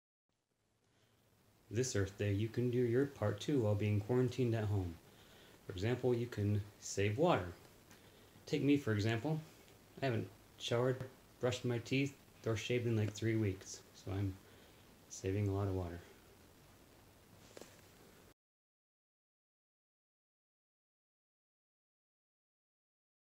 Earth Day with EGLE - Save Water
Jeremy Klein lets out his inner comedian and gives us tips on how to save water.